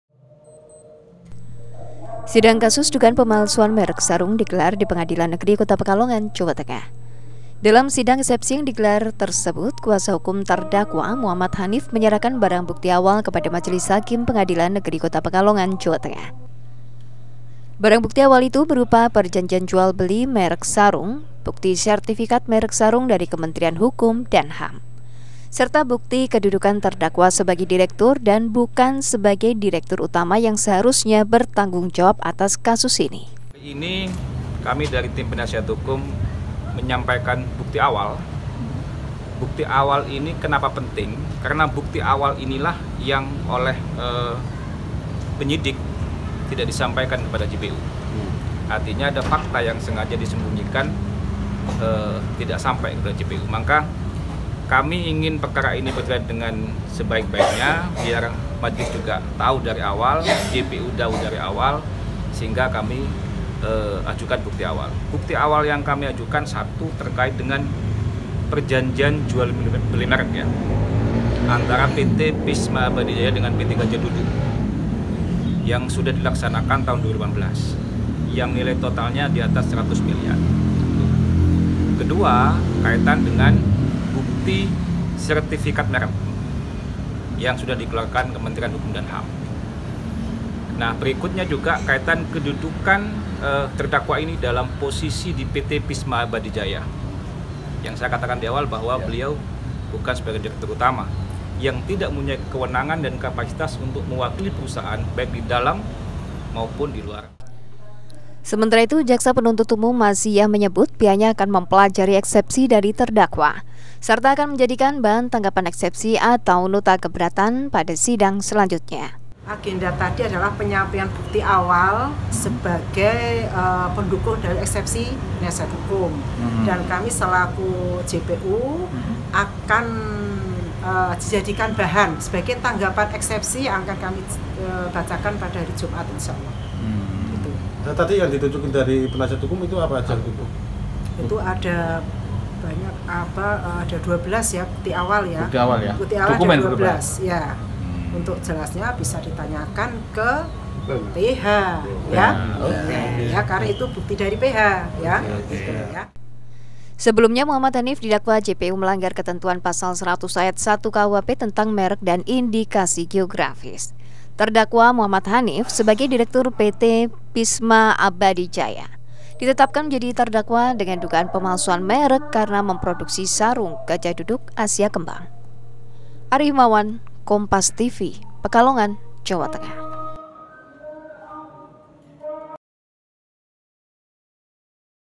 Sidang Dugaan Pemalsuan Merek Sarung, Kuasa Hukum Serahkan Bukti Awal
PEKALONGAN, KOMPAS.TV - Sidang kasus dugaan pemalsuan merek sarung digelar di Pengadilan Negeri Kota Pekalongan, Jawa Tengah. Dalam sidang ekspesi yang digelar Rabu (17/05/2023)siang, kuasa hukum terdakwa Mohammad Khanif menyerahkan barang bukti awal kepada majelis hakim Pengadilan Negeri Kota Pekalongan, Jawa Tengah.

Barang bukti awal itu berupa perjanjian jual beli merek sarung, bukti sertifikat merek sarung dari Kementerian Hukum dan HAM. Serta bukti kedudukan terdakwa sebagai direktur, dan bukan sebagai direktur utama (dirut) yang seharusnya bertanggungjawab atas kasus ini.

"Kami dari tim penasehat hukum menyampaikan bukti awal. Bukti awal ini penting karena dari penyidik tidak sampai ke JPU, " kata Suryono Pane, kuasa hukum terdakwa.

Sementara itu Jaksa Penuntut Umum (JPU), Maziyah menyebut pihaknya akan mempelajari eksepsi dari terdakwa. Serta akan menjadikan bahan tanggapan eksepsi atau nota keberatan pada sidang selanjutnya.

"Agenda tadi adalah penyampaian bukti awal sebagai pendukung dari eksepsi penasehat hukum. Kami selaku JPU akan menjadikan sebagai bahan tanggapan eksepsi," kata Maziyah, JPU.

Sebelumnya Mohammad Khanif didakwa JPU melanggar ketentuan pasal 100 ayat 1 KUHP tentang merek dan indikasi geografis. Terdakwa Mohammad Khanif sebagai Direktur PT Pisma Abadi Jaya, ditetapkan menjadi terdakwa dengan dugaan pemalsuan merek karena memproduksi sarung Gajah Duduk Asia Kembang.